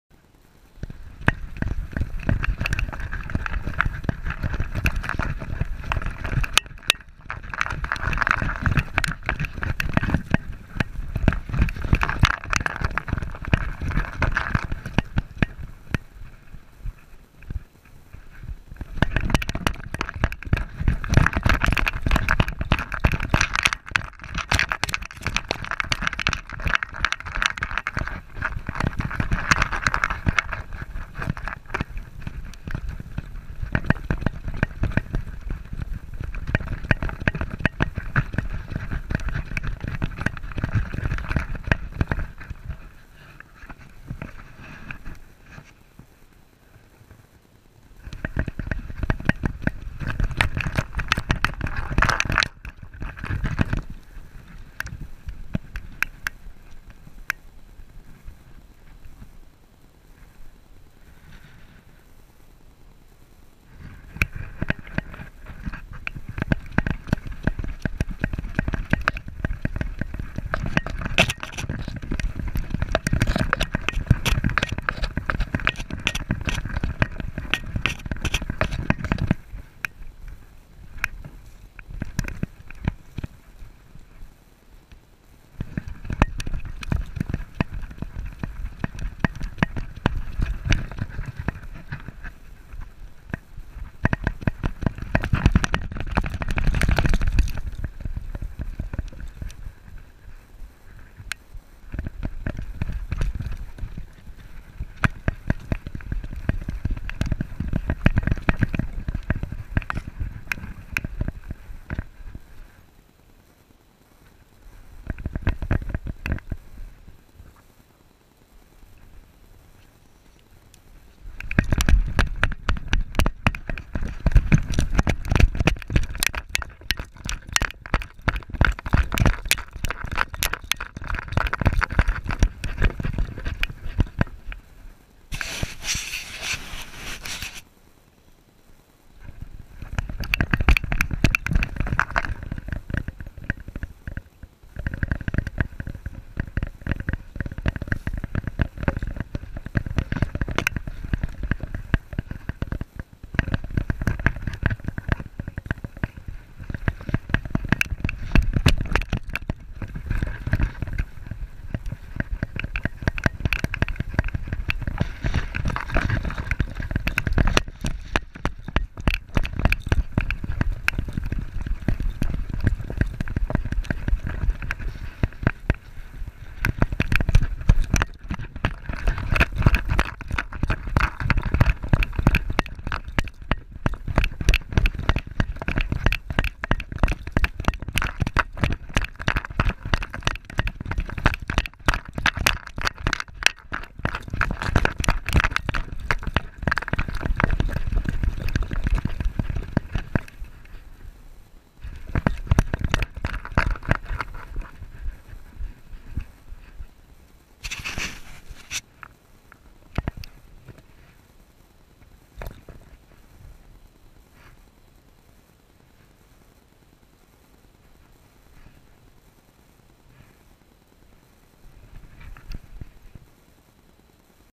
CorgiPRO series: Dobby at Goblin Valley
Goblin valley 11.25.14.  Dobby frapping at Goblin Valley with the CorgiPRO!